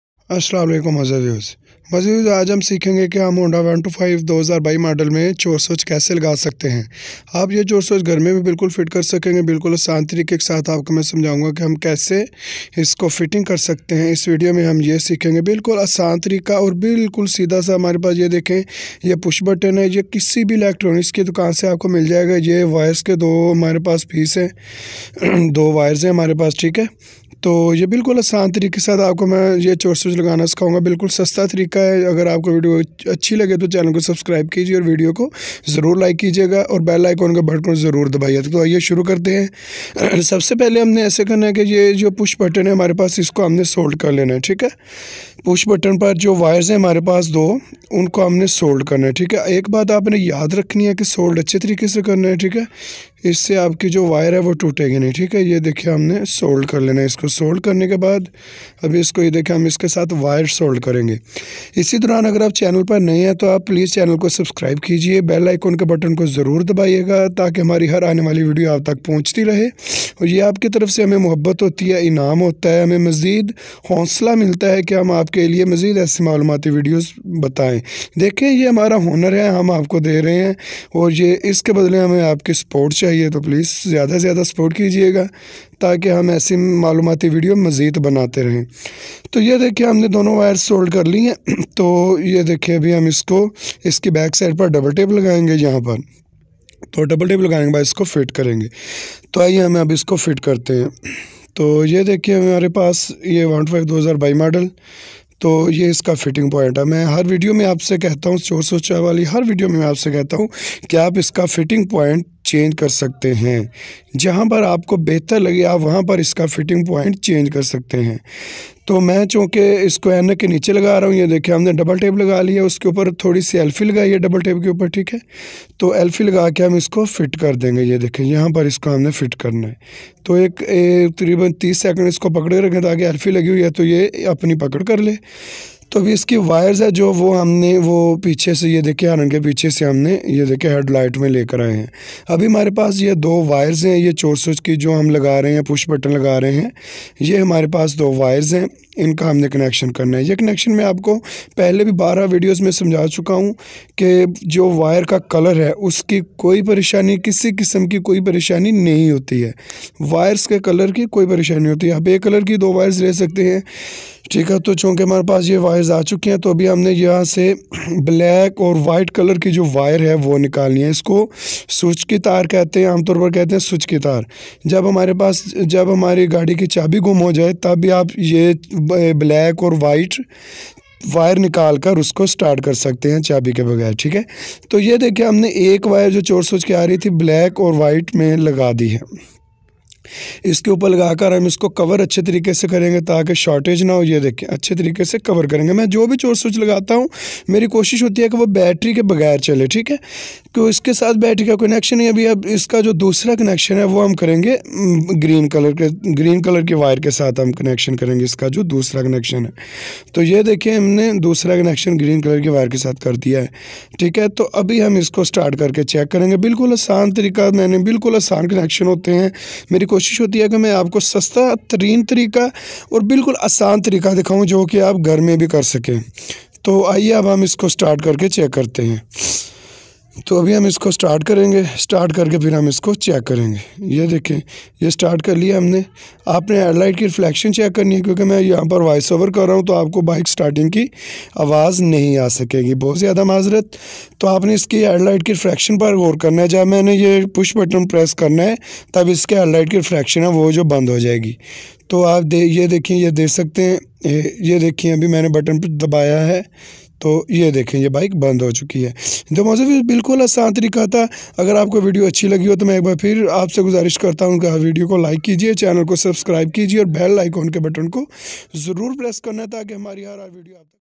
Chor soch lagany ka tarika || Secret switch fitting || chorsoch || honda125 2022
Hello  friends aaj eik baar phir mein hazir hua hu chor soch ki video k saath ajj hum honda 125 2022 model mein choe soch lgana seekhin gy wo bhui bilkl asan teiky k saath k aap ghar par bhi fitting kar sakty hain aaiy dekhty hein vidoeo
honda 125 2022 model